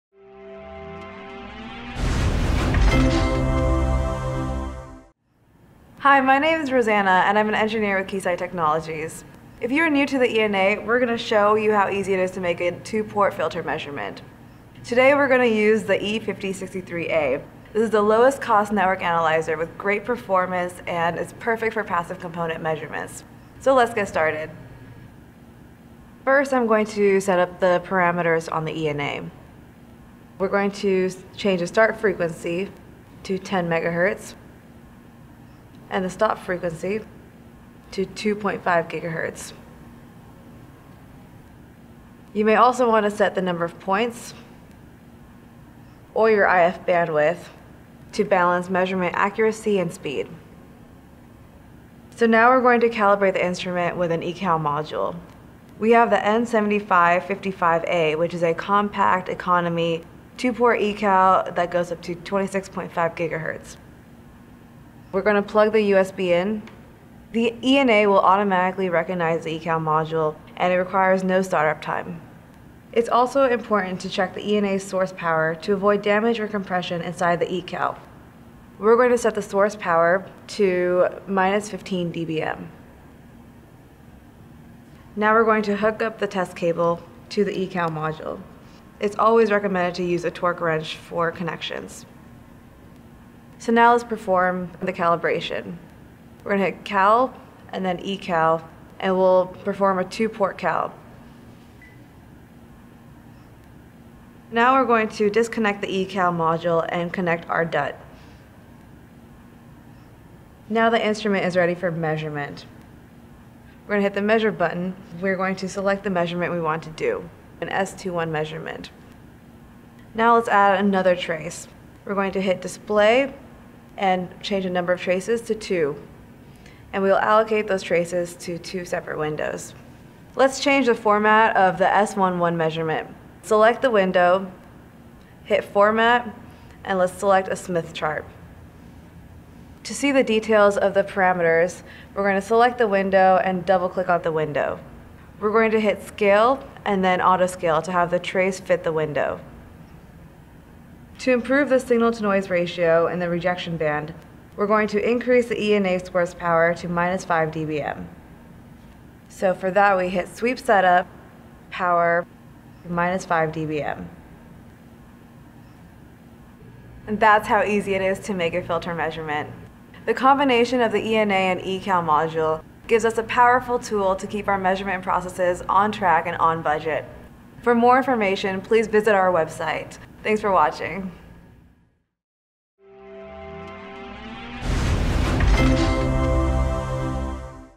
Two-port filter measurement | ENA network analyzer
See how easy it is to make a two-port filter measurement.

This demonstration uses a Keysight E5063A ENA network analyzer and Keysight N7555A ECal to perform a quick and easy two-port filter measurement.